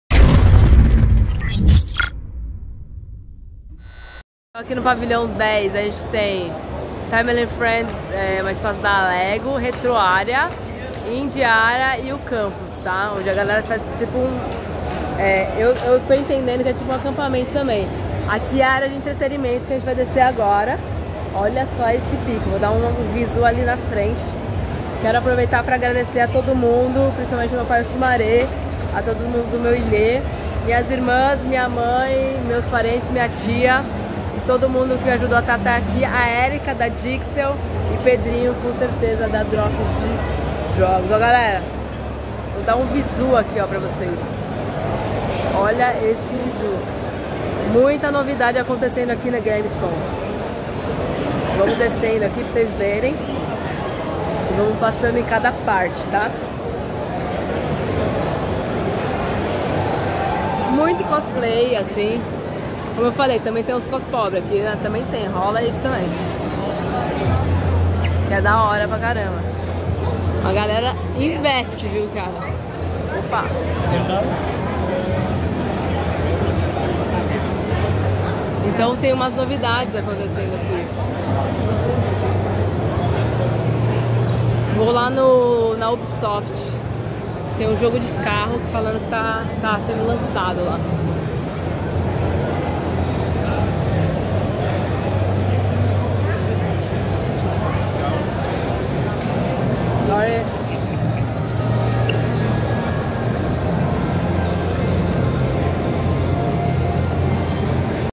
Drops de Jogos na Gamescom 2023: Conheça a área de Entretenimento do evento
Com Amanda Pellini.